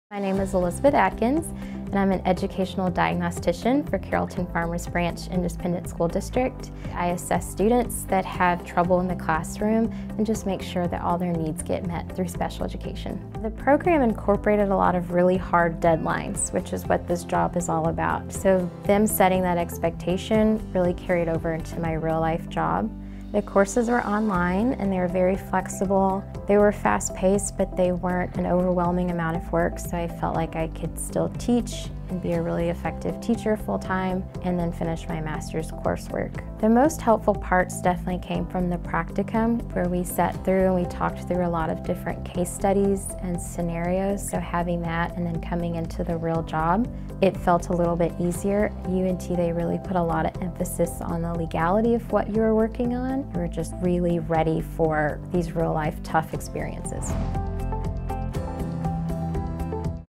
Educational Diagnostician
Elizabeth Atkins earned a Master's Degree in Special Education with a concentration in Educational Diagnostician from the University of North Texas and works helping to identify learning needs of students.